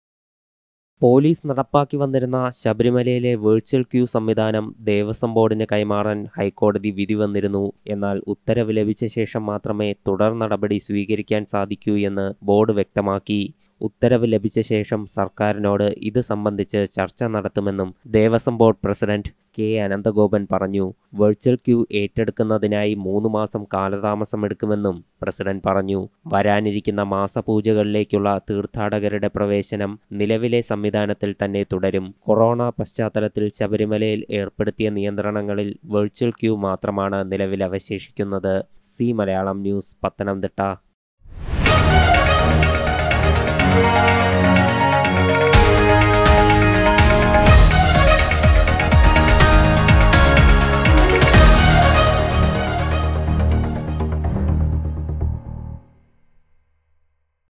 Sabarimala | ശബരിമലയിലെ വിർച്വൽ ക്യു സംവിധാനം ഏറ്റെടുക്കാൻ 3 മാസം വേണമെന്ന് ദേവസ്വം ബോർഡ്
ശബരിമലയിലെ വിർച്വൽ ക്യു സംവിധാനം ഏറ്റെടുക്കാൻ  3 മാസം വേണമെന്ന് ദേവസ്വം ബോർഡ്.